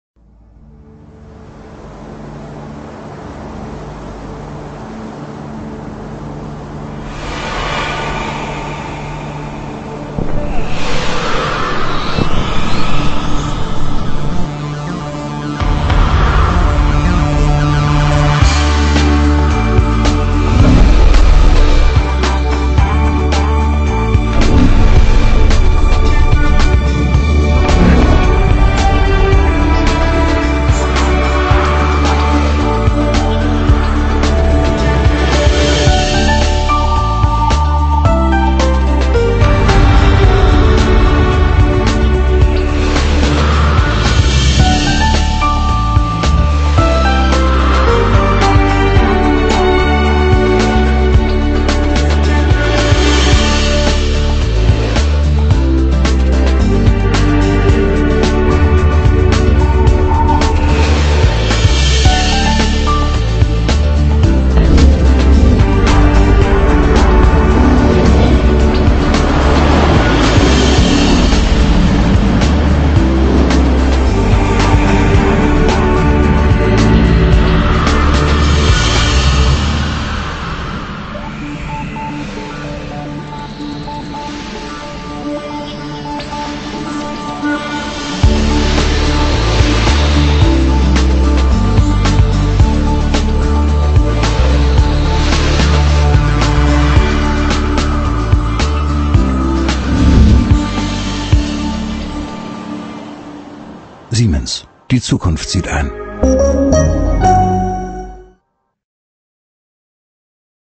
Siemens blueTherm Wärmepumpentrockner / Heat Pump Dryer
This is the same dryer as the Bosch EcoLogixx 7 - but with the Siemens name on it. Different video, however!